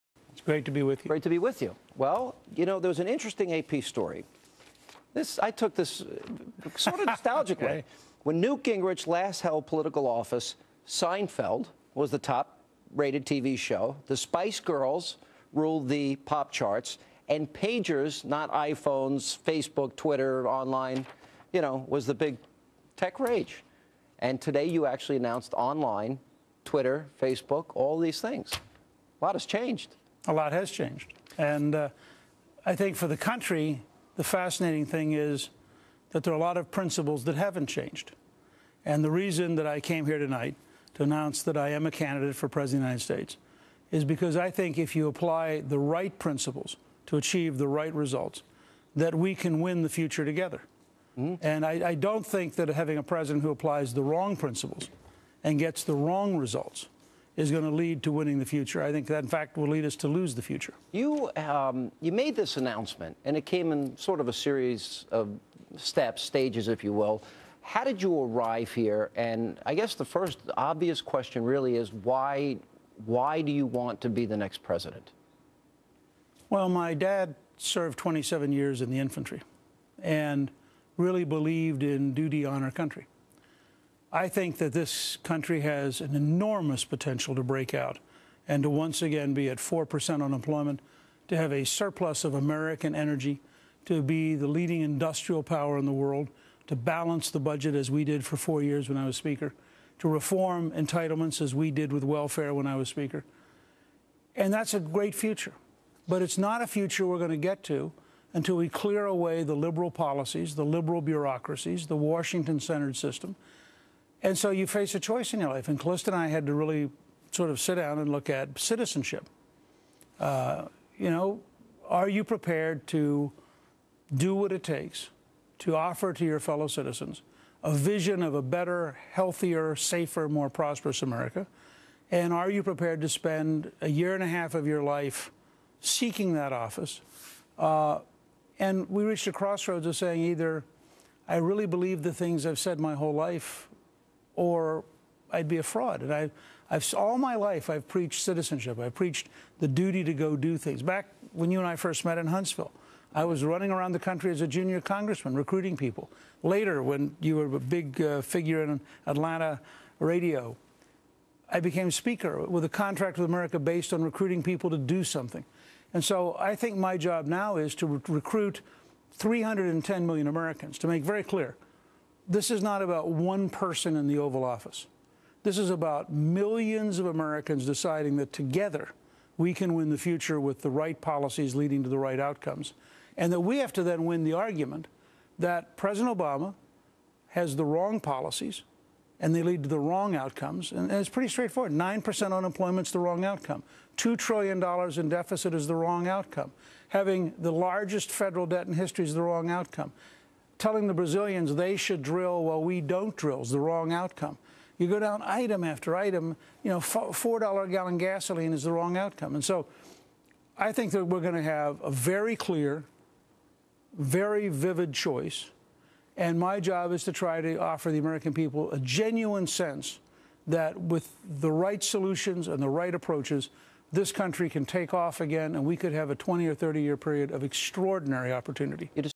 Newt Gingrich Like Him Or Hate Him Running For President Part I
Newt Gingrich announced he is running for president.  Many like Newt and many hate Newt.  He says, "Apply the right principles and you get the right results."  He also said Obama has applied the wrong principles and gotten the wrong results.